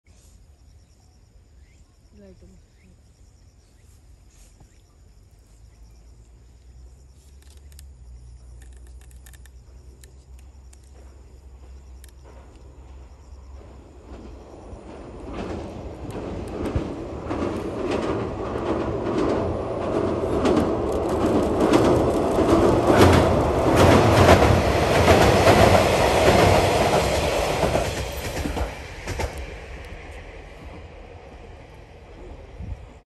キハ47系 いさぶろう・しんぺい 高城〜鶴崎
上げ忘れです。
この日は団臨運転だったそうです。

いさぶろう・しんぺい